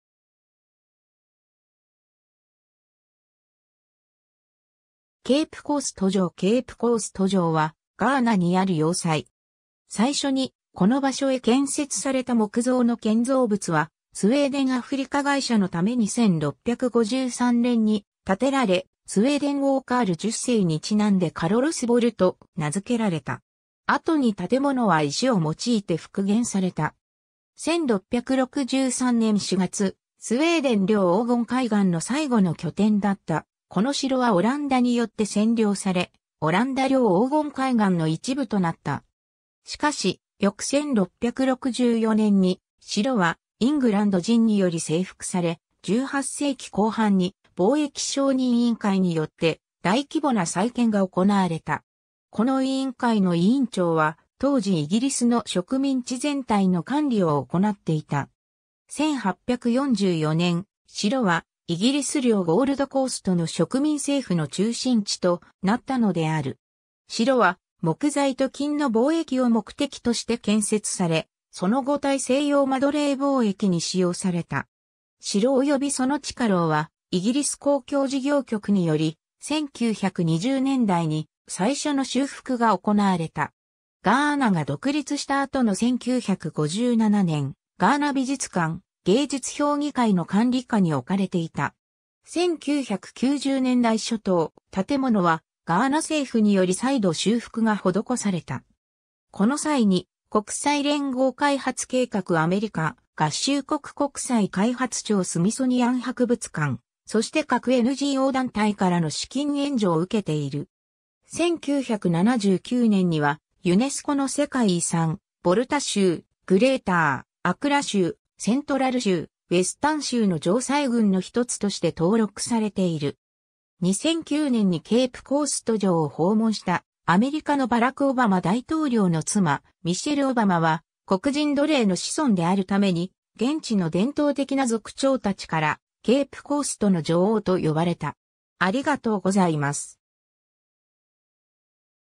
ケープ・コースト城
ケープ・コースト城 ケープ・コースト城（英文表記: Cape Coast Castle）は、ガーナにある要塞。
最初にこの場所へ建設された木造の建造物は、スウェーデン・アフリカ会社のために1653年に建てられ、スウェーデン王カール10世にちなんでカロルスボル (Carolusborg) と名付けられた。
後に建物は石を用いて復元された。
 1663年4月、スウェーデン領黄金海岸の最後の拠点だったこの城はオランダによって占領され、オランダ領黄金海岸の一部となった。
しかし翌1664年に、城はイングランド人により征服され、18世紀後半に貿易商人委員会 (Committee of Merchants) によって大規模な再建が行われた。
この委員会の委員長は、当時イギリスの植民地全体の管理を行っていた。
1844年、城はイギリス領ゴールド・コーストの植民政府の中心地となったのである。
 城は木材と金の貿易を目的として建設され、その後大西洋間奴隷貿易に使用された。
城及びその地下牢はイギリス公共事業局により、1920年代に最初の修復が行われた。
ガーナが独立した後の1957年、ガーナ美術館・芸術評議会（Ghana Museums and Monuments Board、略称GMMB）の管理下に置かれていた。
1990年代初頭、建物はガーナ政府により再度修復が施された。
この際に国際連合開発計画 (UNDP) 、アメリカ合衆国国際開発庁 (USAID) 、スミソニアン博物館、そして各NGO団体からの資金援助を受けている。
 1979年にはユネスコの世界遺産「ヴォルタ州、グレーター・アクラ州、セントラル州、ウェスタン州の城塞群」の一つとして登録されている。
 2009年にケープ・コースト城を訪問したアメリカのバラク・オバマ大統領の妻ミシェル・オバマは黒人奴隷の子孫であるために、現地の伝統的な族長たちから「ケープ・コーストの女王」と呼ばれた。